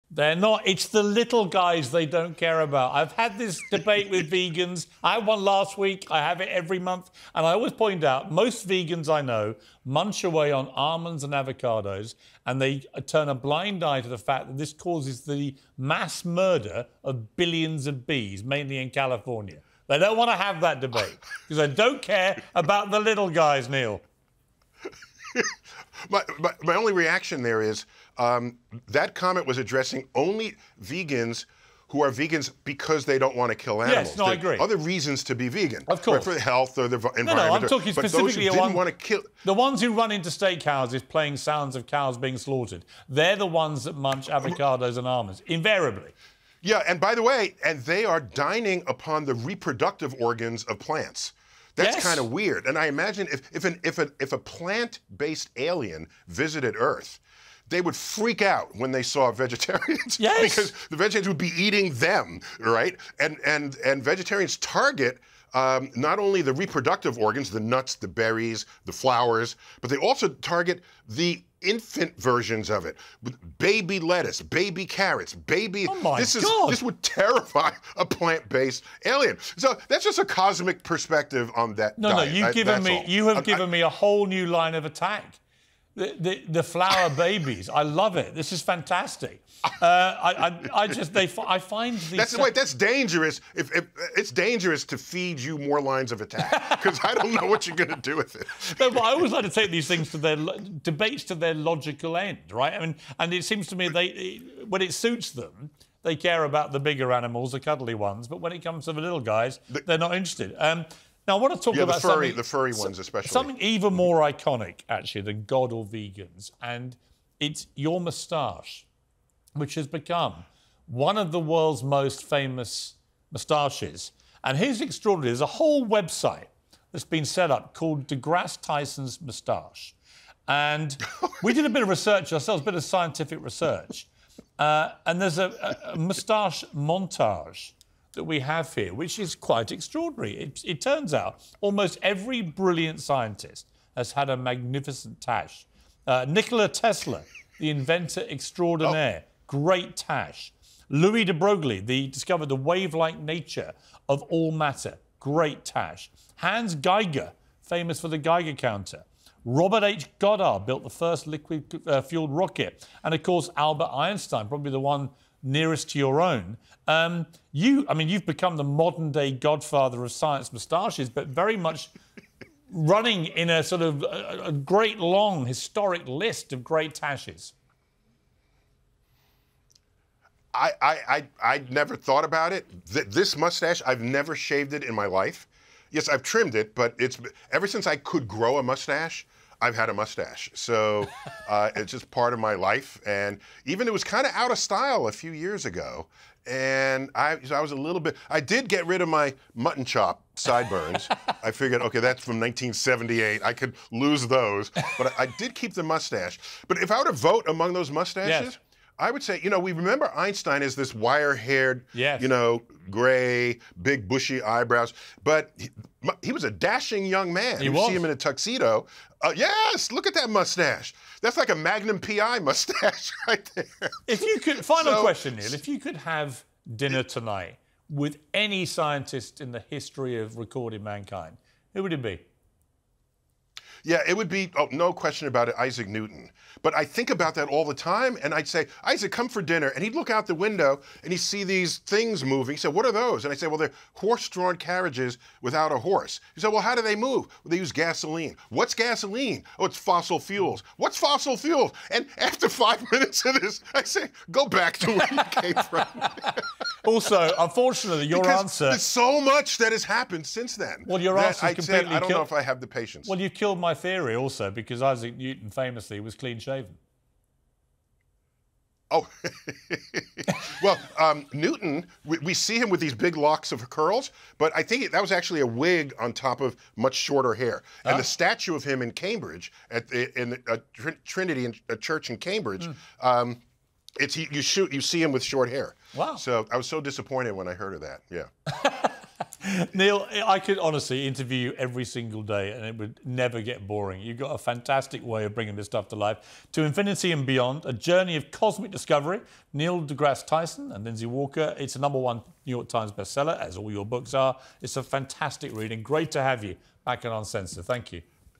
Piers Morgan proves ‘every’ great scientist had a moustache
Sky News Australia host Piers Morgan presents his “moustache montage” of the biggest names in science.

“We did a bit of research ourselves,” Mr Morgan said.

He said it turned out “every” brilliant scientist has had a “magnificent tache”.

“Nikola Tesla, the inventor extraordinaire – great tache. Louis De Broglie, he discovered the wave-like nature of all matter – great tache.

“Hans Geiger, famous for the Geiger counter. Robert H Goddard, built the liquid fuelled rocket, and of course Albert Einstein.”